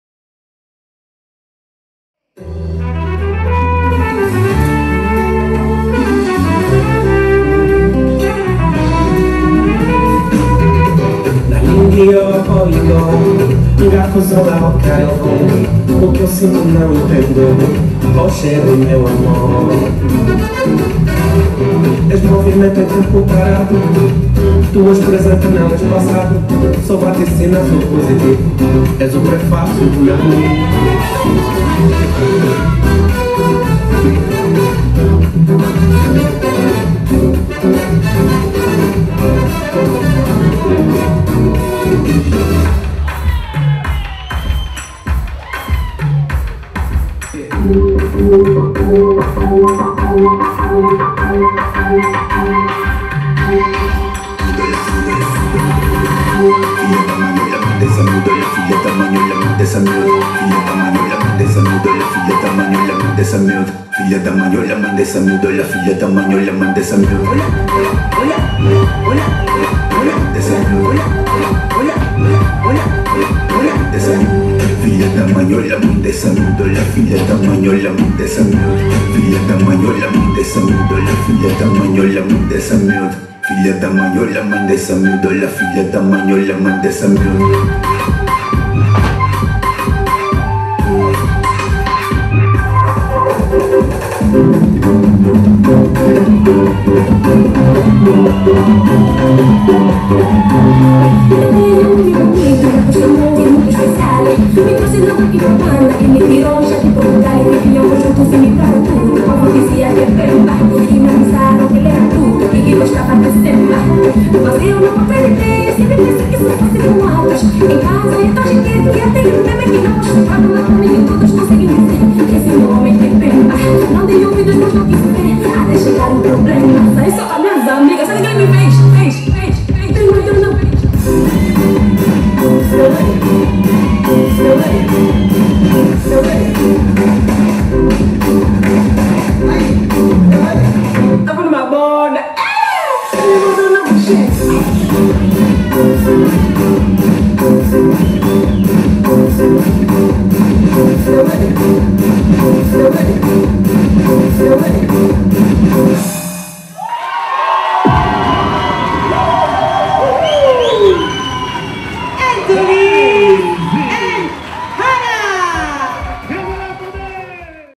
Salpicon Latino - ANTHONY & HARA
Sul palco del HOTEL MIDAS, sabato 14 ottobre 2017, i vincitori dell'AFRICAN DANCAR 2015, la coppia greca formata da ANTHONY & HARA, si esibiscono in una loro coreografia di semba e kizomba durante lo show del ROMA KIZOMBA FESTIVAL 2017, organizzato da KIZOMBA ROMANA.